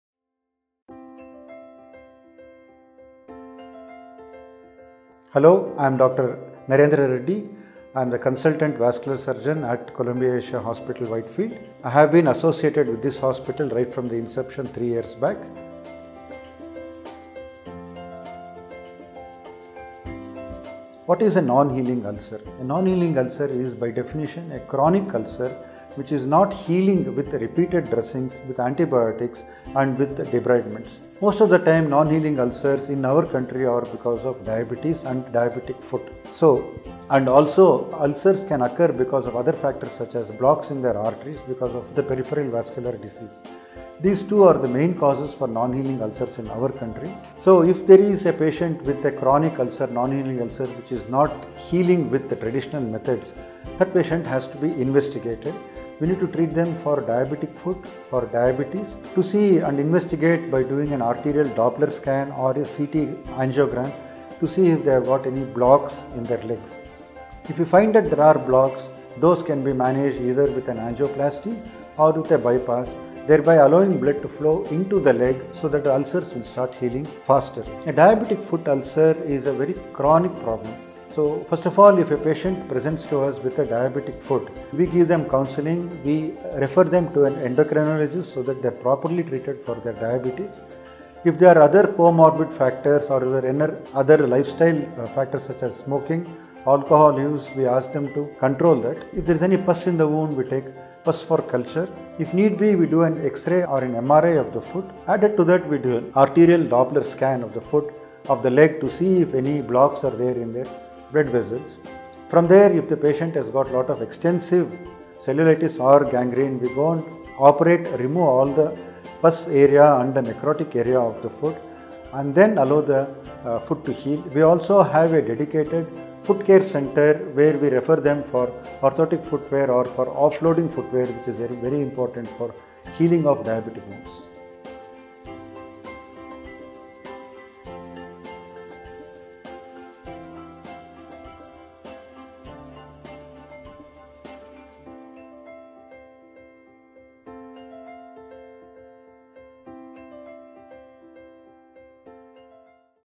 What are types of non healing ulcers & How should a diabetic foot ulcer be treated?
Dr. Narendra Kumar Reddy - Consultant - Vascular Surgery, Columbia Asia Hospital - Whitefield